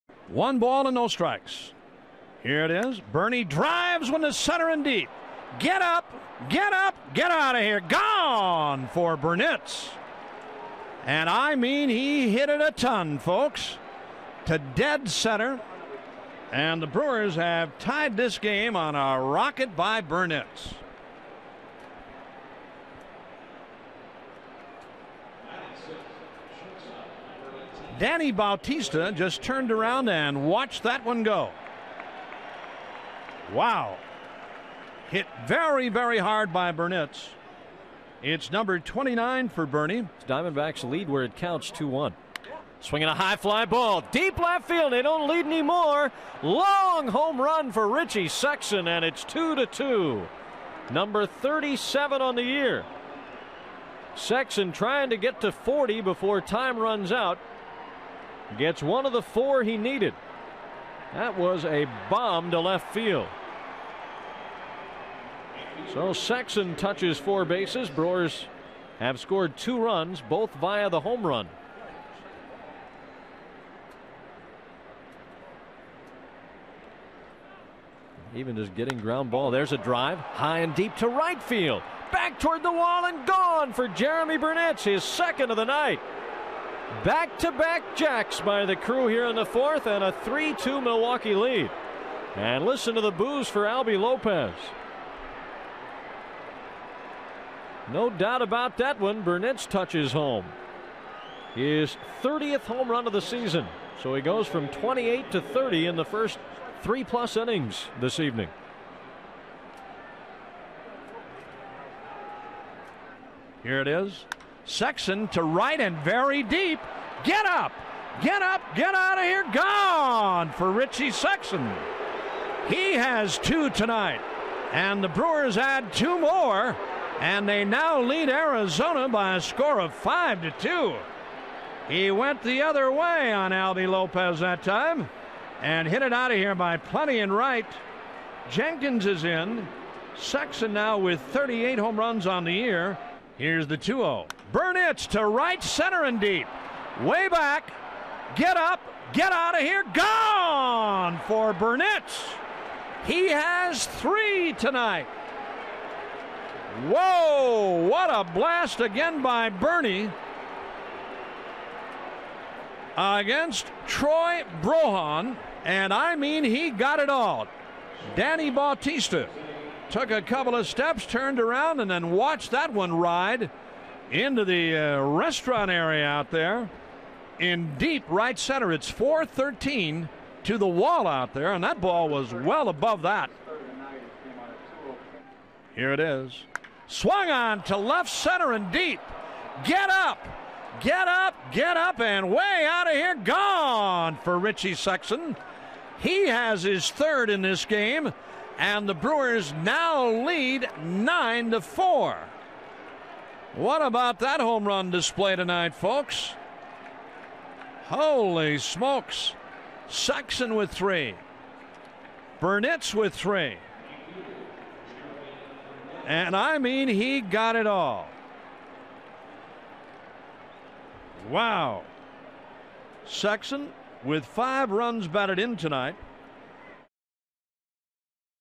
MIL@ARI: Burnitz, Sexson each homer 3 times
9/25/01: Brewers teammates Jeromy Burnitz and Richie Sexson each homer three times against the Diamondbacks